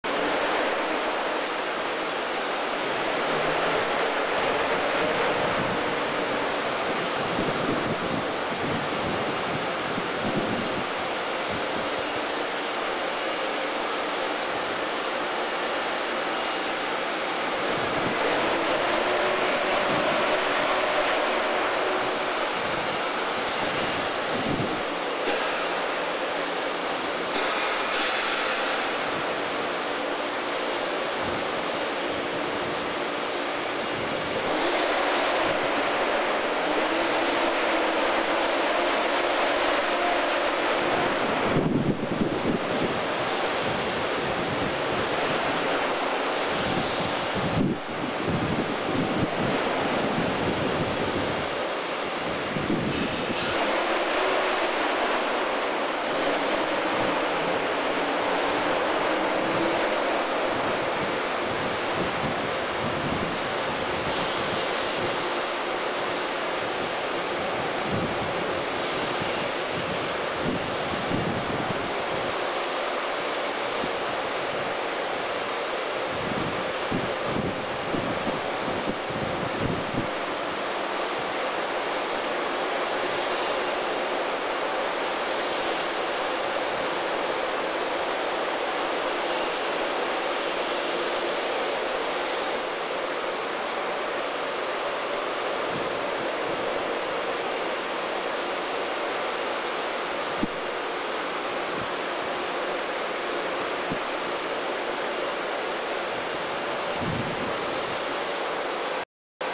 JASDF F-4EJ Phantom II close up.
Here's an up close video of a F-4EJ in the hangar that the Japanese provided us space in for our two week det to Komatsu. Filmed in 1991 by me during VMFA-531's final deployment before being decommissioned in 1992.